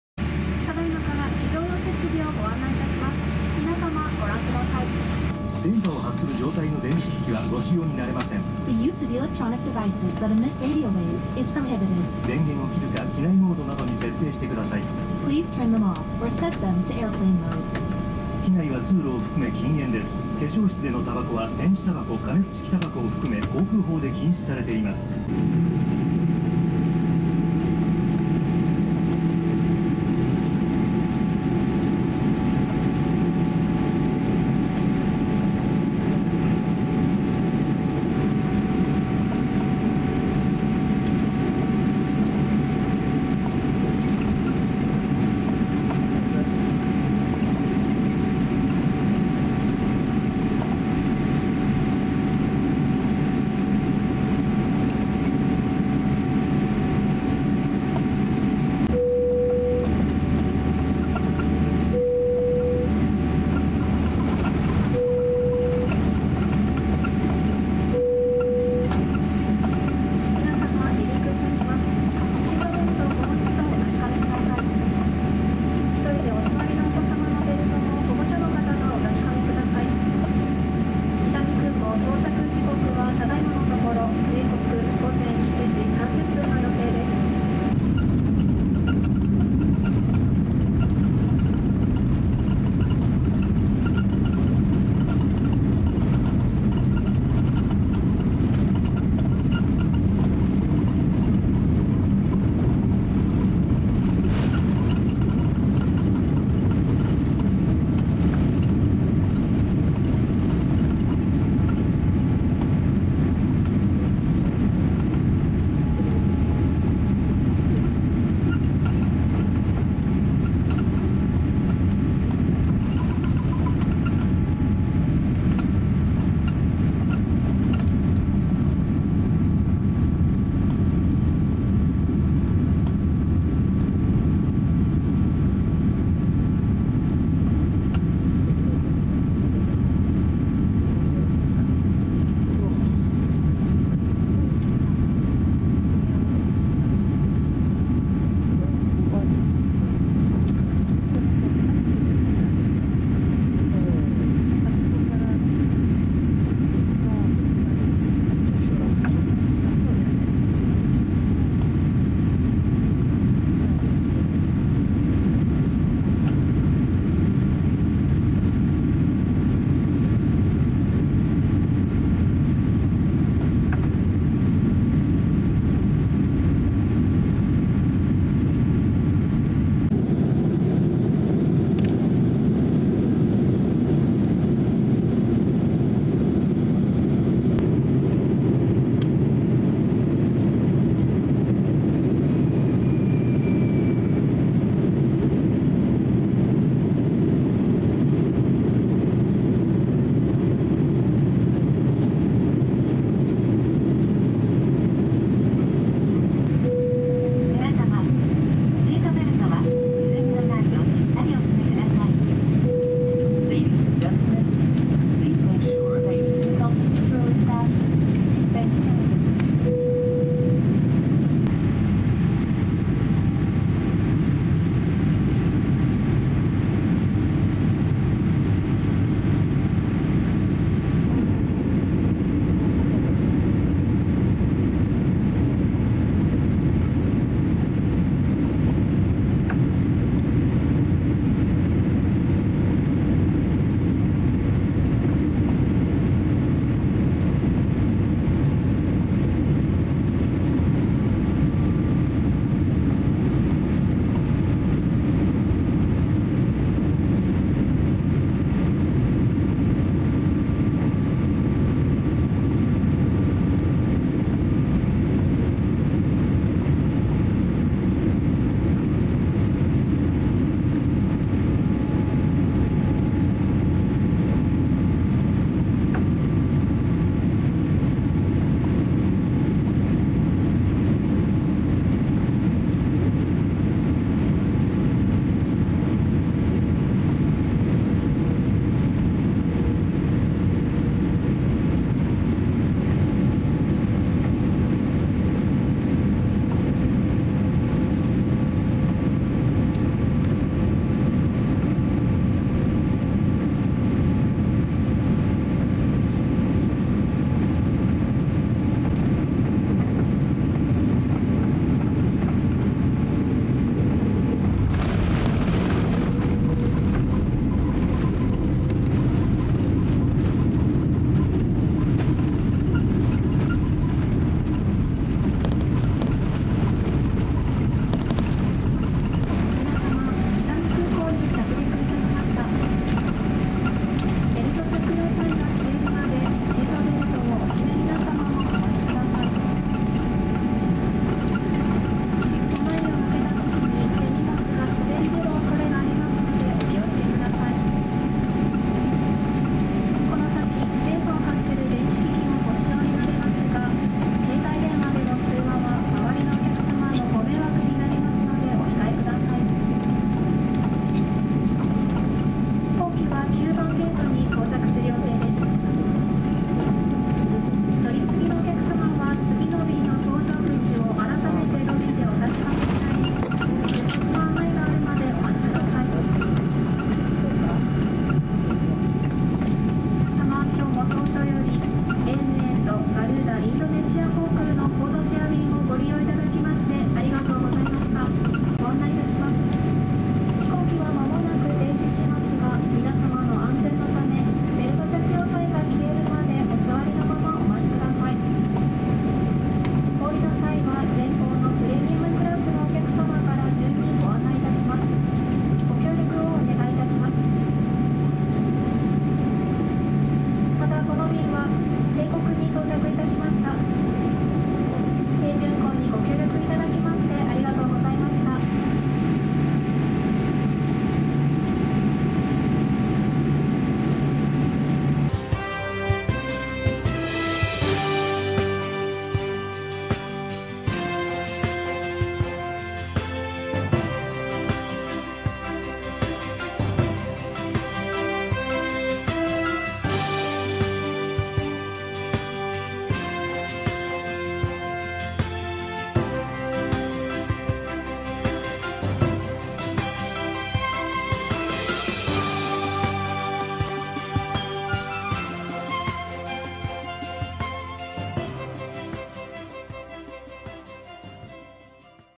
【ANAダイジェスト】ANA985便 羽田空港→大阪伊丹空港
2022年7月17日、ANA985便、羽田空港→大阪伊丹空港Boeing787- 8(JA819A)より撮影した羽田空港A滑走路離陸～大阪高層ビル～大阪空港着陸ダイジェスト・ハイライト映像です。

Everyday Update!